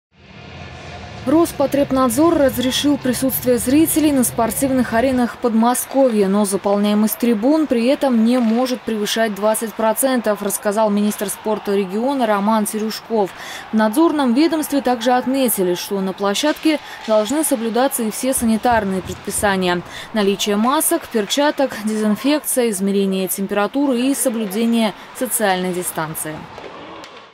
Роспотребнадзор разрешил присутствие зрителей на спортивных аренах Подмосковья
Заполняемость площадки не должна превышать 20%